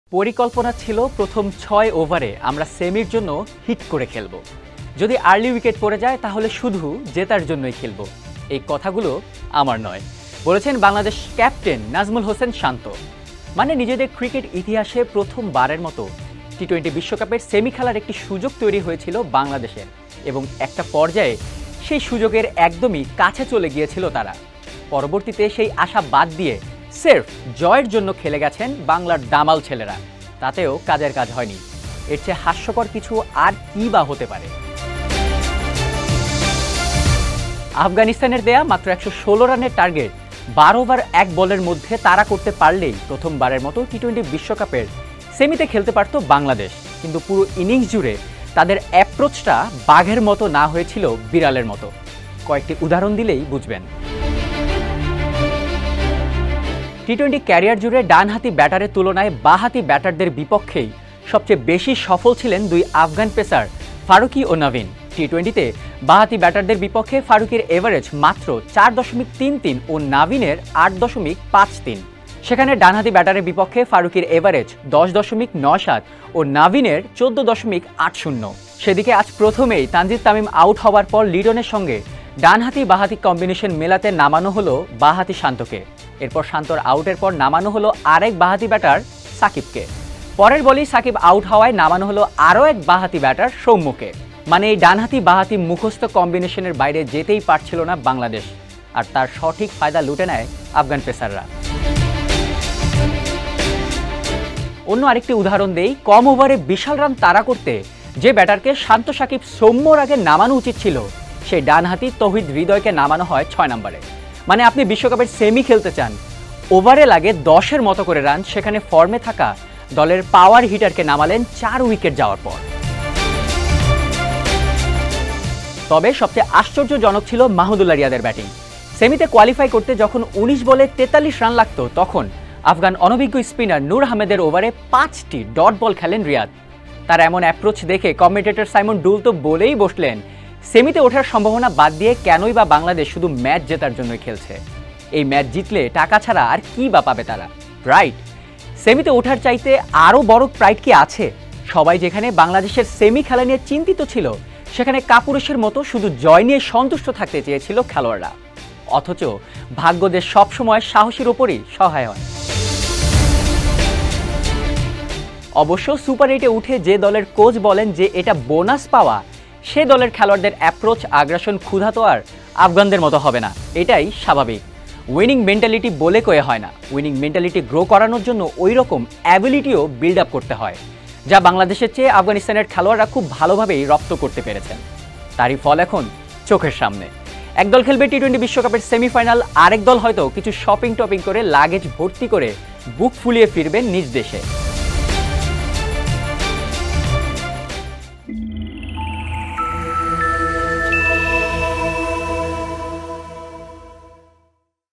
কমনসেন্সের অভাব, নাকি কোন সেন্সই ছিল না বাংলাদেশের? | Bangladesh Cricket | TBS Sports
Is a lack of common sense ailing Bangladesh cricket?
আফগানিস্তানের দেয়া মাত্র ১১৬ রানের টার্গেট ১২ ওভার ১ বলের মধ্যে তাড়া করতে পারলেই প্রথমবারের মত টি২০ বিশ্বকাপের সেমিতে খেলতে পারতো বাংলাদেশ। কিন্তু পুরো ইনিংস জুড়ে তাদের অ্যাপ্রোচটা বাঘের মত না হয়ে ছিল বিড়ালের মত। কয়েকটি উদাহরণ দিলেই বুঝবেন।